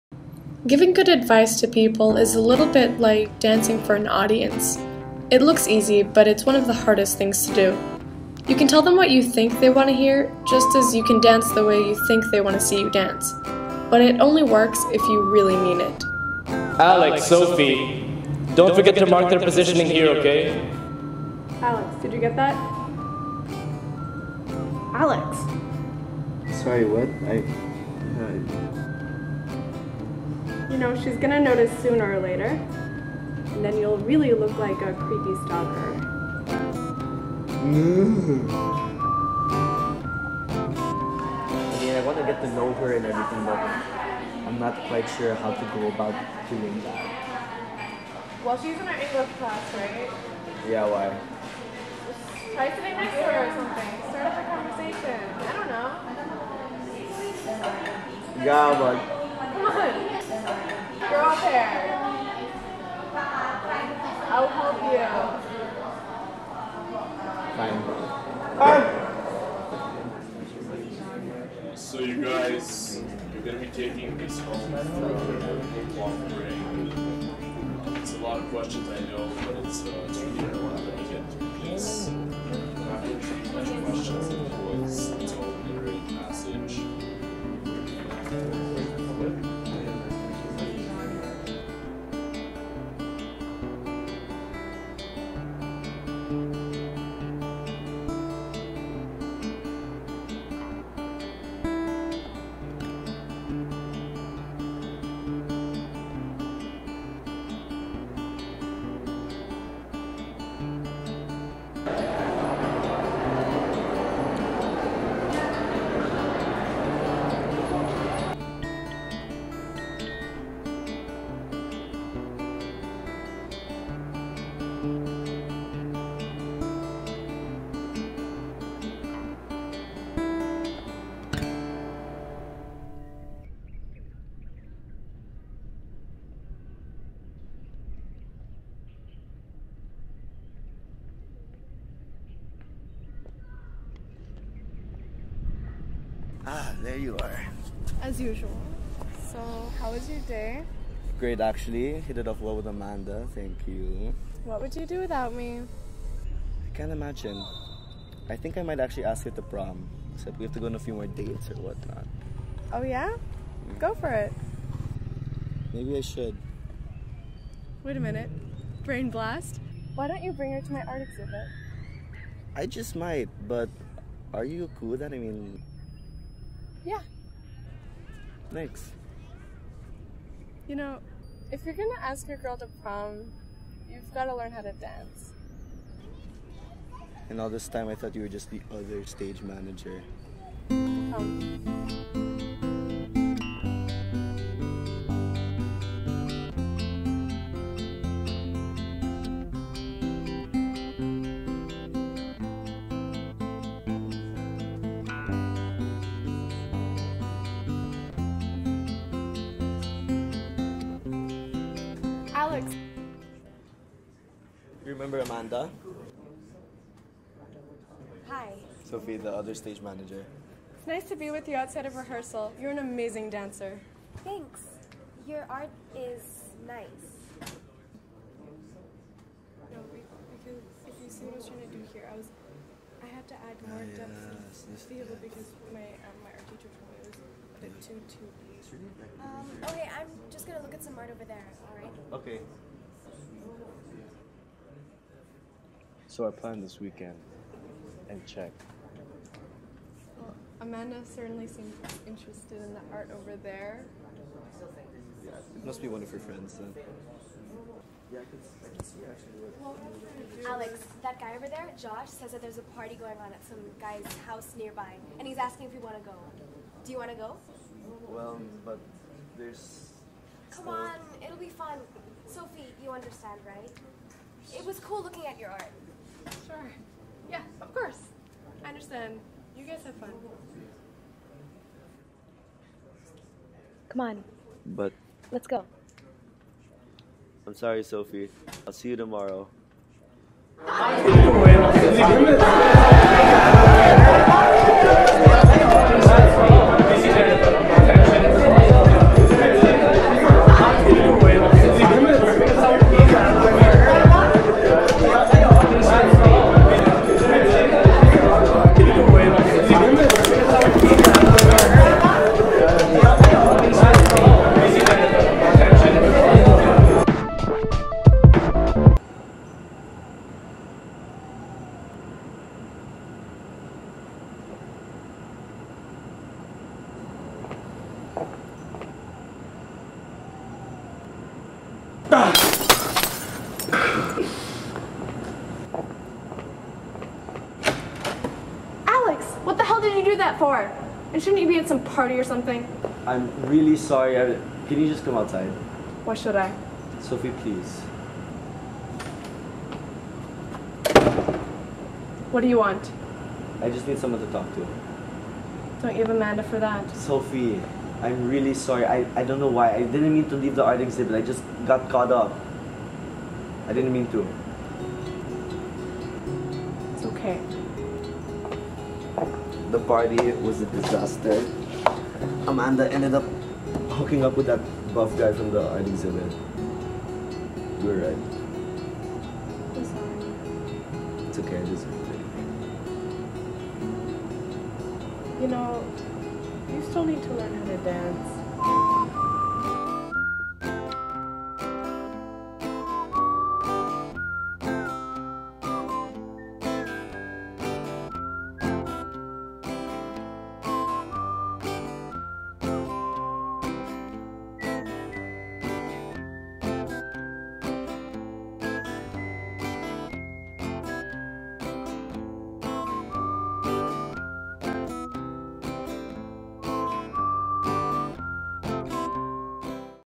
ALEX & SOPHIE (2011)
this short film was my final production for IB Higher Level Film as a senior in high school.

it tells the story of two best friends whose friendship is jeopardized by a third party love interest